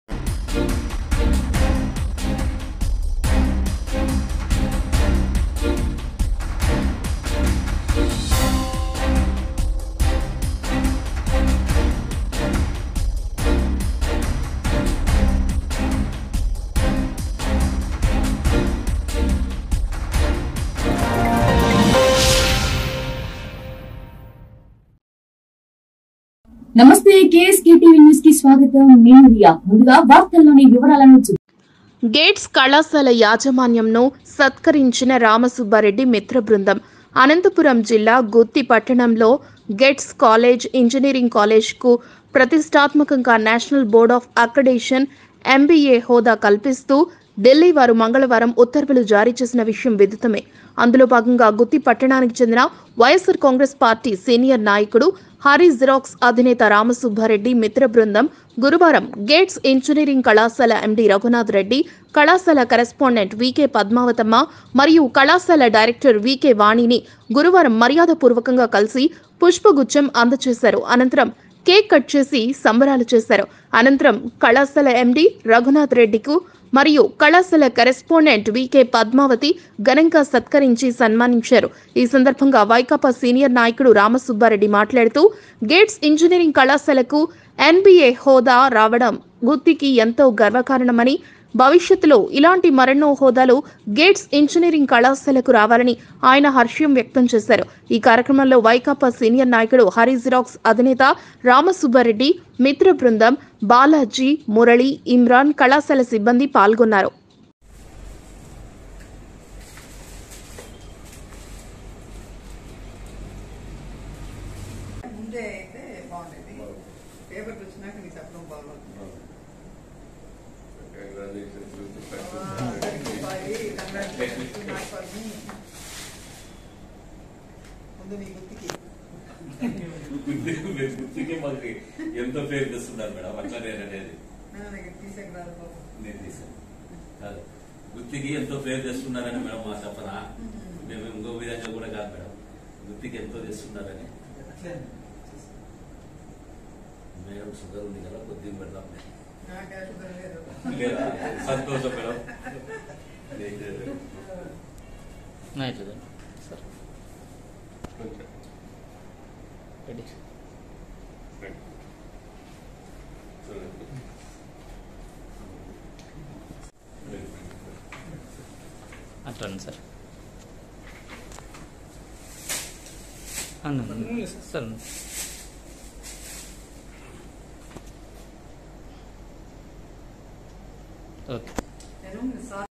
గేట్స్ కళాశాల యాజమాన్యమును సత్కరించిన రామ సుబ్బారెడ్డి మిత్ర బృందం.
అనంతపురం జిల్లా గుత్తి పట్టణంలో గేట్స్ ఇంజనీరింగ్ కళాశాలకు ప్రతిష్టాత్మకంగా నేషనల్ బోర్డ్ ఆఫ్ అక్రిడేషన్ ఎంబీఏ హోదా కల్పిస్తూ ఢిల్లీ వారు మంగళవారం ఉత్తర్వులు జరీచేసిన విషయం విధితమే.అందులో భాగంగా గుత్తి పట్టడానికి చెందిన వైకాపా సీనియర్ నాయకుడు,హరి జిరాక్స్ అధినేత రామ సుబ్బారెడ్డి మిత్రబృందం గురువారం గేట్స్ ఇంజనీరింగ్ కళాశాల ఎండి రఘునాథ్ రెడ్డి, కళాశాల కరస్పాండెంట్ వికె పద్మావతమ్మ, మరియు కళాశాల డైరెక్టర్ వీకే వాణి ని గురువారం మర్యాదపూర్వకంగా కలిసి పుష్ప గుచ్చం అందజేశారు.అనంతరం కేక్ కట్ చేసి సంబరాలు చేశారు.అనంతరం కళాశాల ఎండి రఘునాథ్ రెడ్డి కు మరియు కళాశాల కరస్పాండెంట్ వీకే పద్మావతి ఘనంగా సత్కరించి సన్మానించారు.ఈ సందర్భంగా వైకాపా సీనియర్ నాయకుడు రామ సుబ్బారెడ్డి మాట్లాడుతూ గేట్స్ ఇంజనీరింగ్ కళాశాలకు ఎన్బీఏ హోదా రావడం గుత్తికి ఎంతో గర్వకారణమని భవిష్యత్తులో ఇలాంటి మరెన్నో హోదాలు గేట్స్ ఇంజనీరింగ్ కళాశాలకు రావాలని ఆయన హర్షం వ్యక్తం చేశారు.ఈ కార్యక్రమంలో వైకాపా సీనియర్ నాయకుడు హరి జిరాక్స్ అధినేత రామ సుబ్బారెడ్డి మిత్రబృందం,బాలాజీ,మురళి,ఇమ్రాన్ ,కళాశాల సిబ్బంది పాల్గొన్నారు.